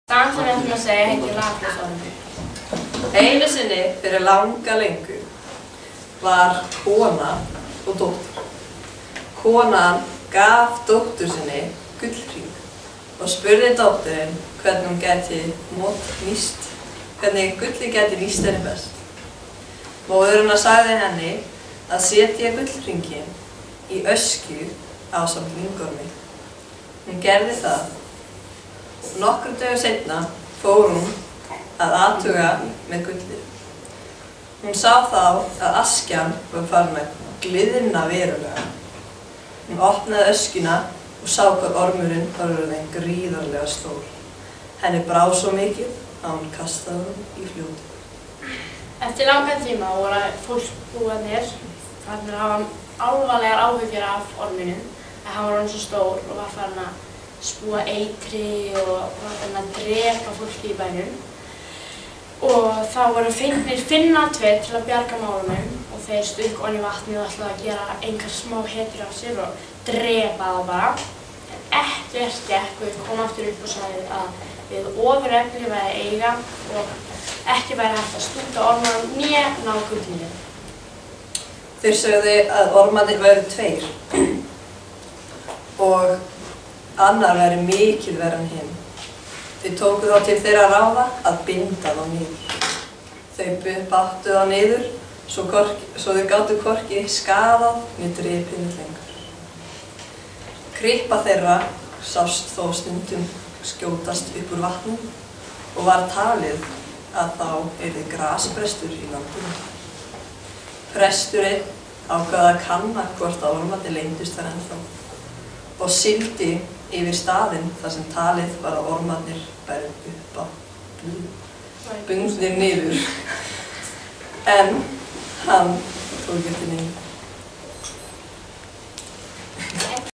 The worm in Lagarfljót - Icelandic folktale.
Lagarfjótsormurinn (The worm in Lagarfljót) is an Icelandic folktale. Here told by students in Blönduskóli during Comenius project (From Local Residents to European Citizens), spring 2013.